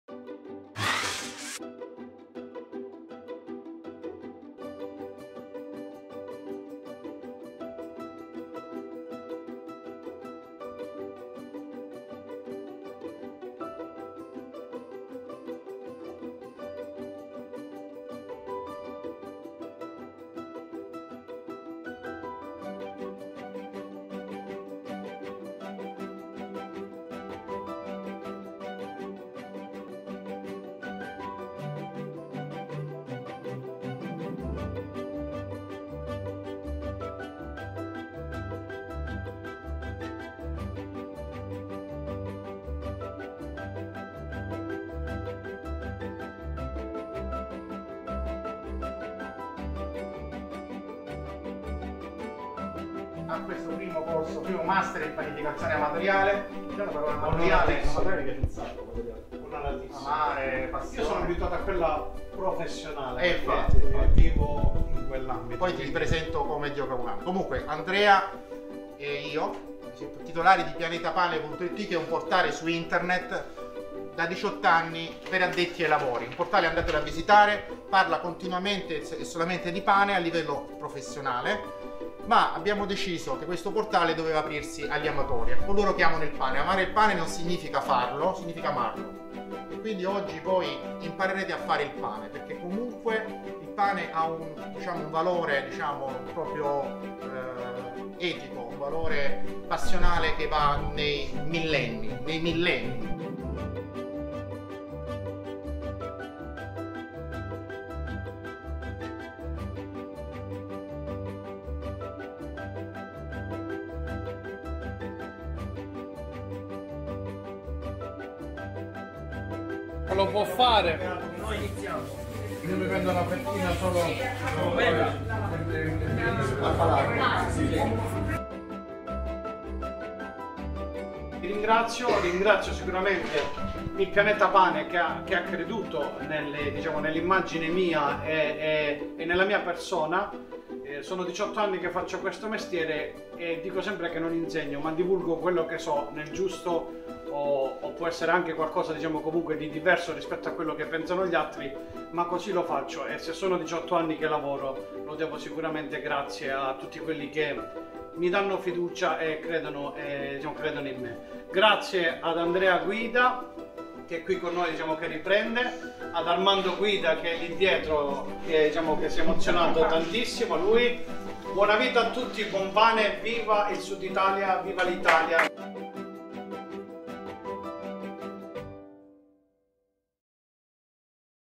Ecco come è andato il 1° Master di Panificazione Amatoriale di Pianetapane.it
Con piacere condividiamo con tutti i lettori di Pianetapane.it, un piccolo estratto delle due giornate di formazione del 1°  Master di Panificazione Amatoriale, tenutosi l'8 e 9 febbraio 2020 a Lecce.
Un'esperienza emozionante e coinvolgente, durante la quale, insieme al Maestro Salvatore Vullo abbiamo trasmesso il Valore del Pane.
Ringraziamo ancora tutti i partecipanti al corso e vi raccomandiamo di rimanere collegati per non perdere tutte le novità in arrivo.